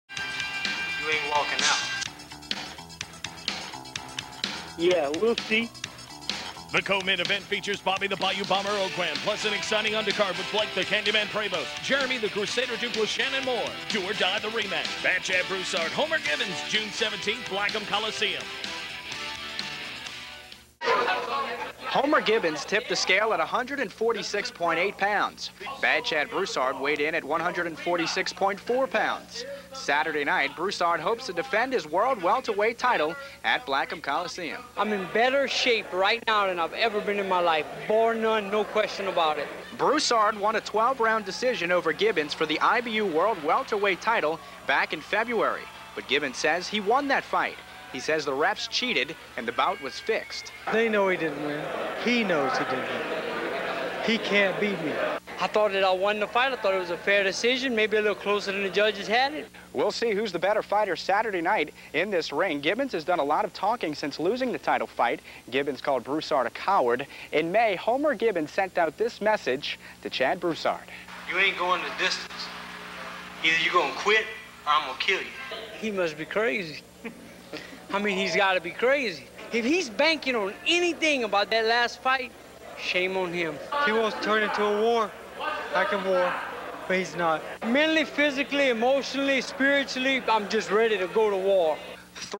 IBU Promo
Bad Chad Broussard Vs Homer Gibbins for the IBU World Boxing Welter-weight title